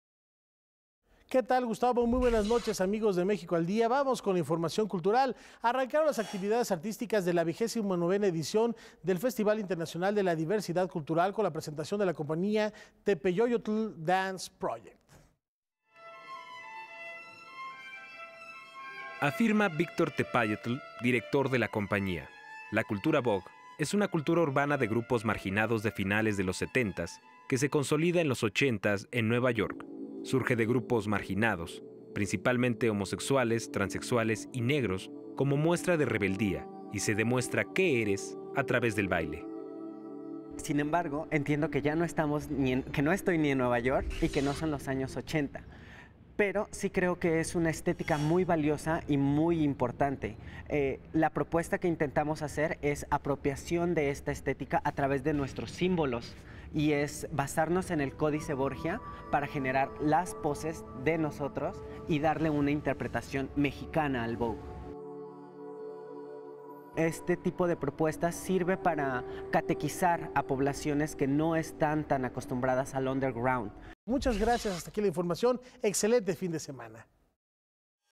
11/06/16 Cultura Canal 22
Rafael García Villegas de Canal 22 México, nos comparte lainformación cultural más relevante:
Tepeyollotl Dance Project formó parte de las actividades de la 29° edición del Festival Internacional por la Diversidad Sexual